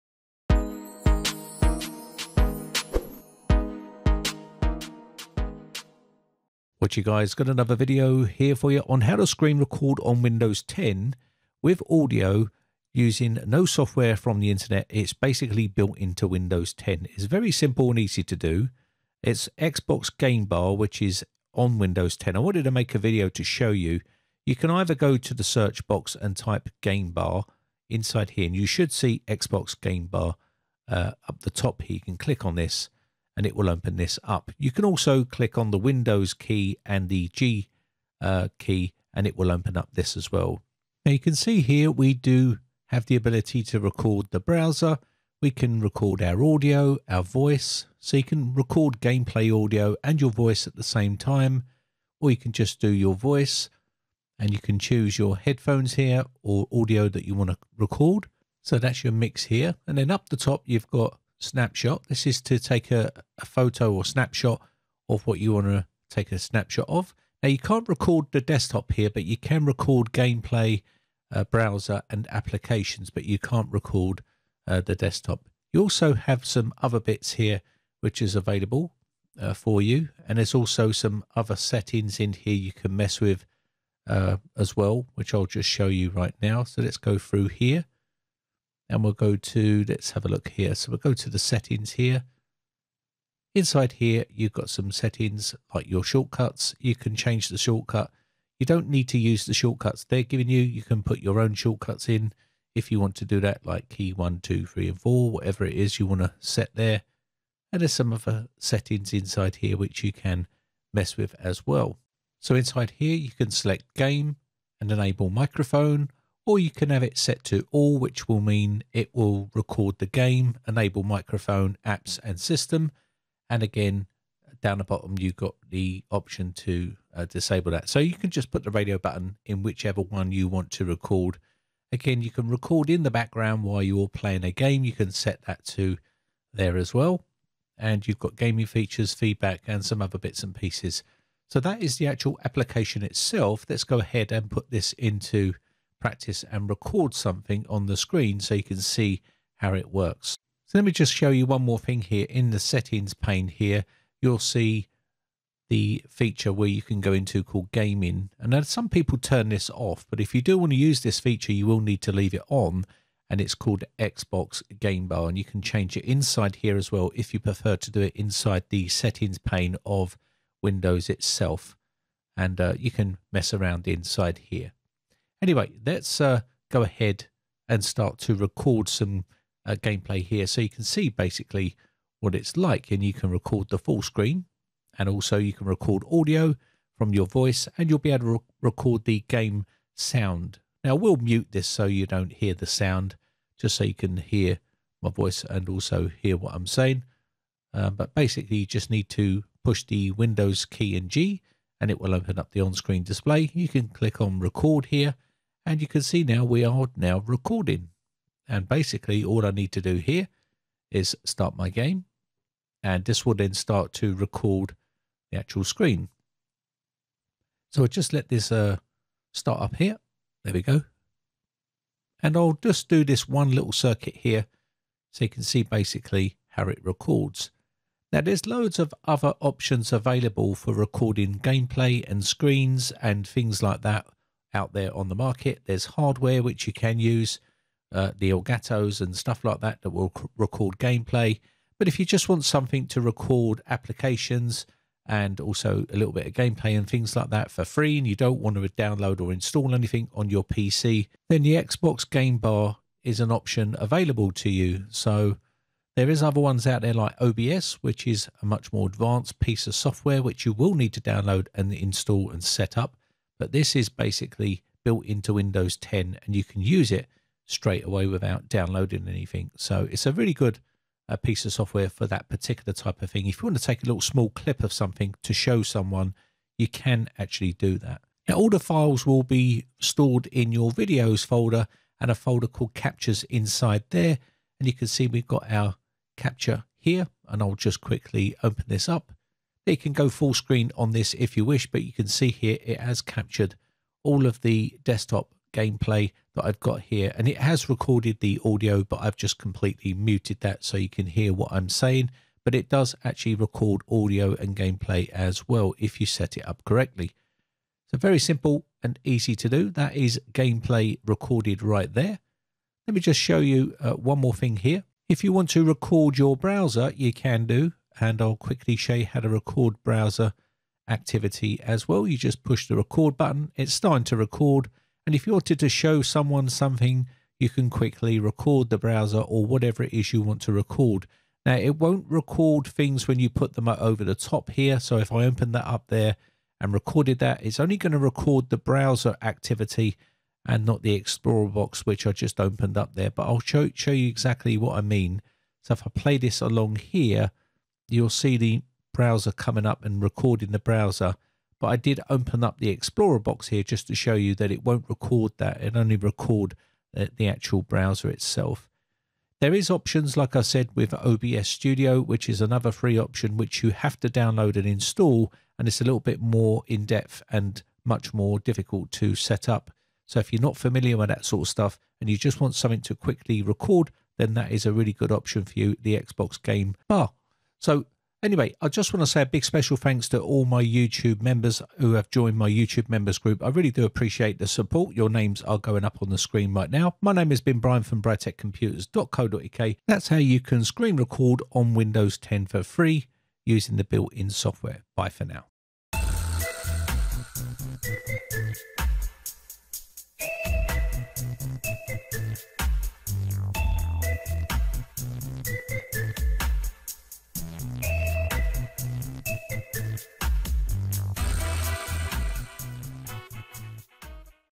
How to Screen Record on Windows 10 With Audio
This is a FREE way to record the screen without downloading any software. Its use Xbox Game Bar.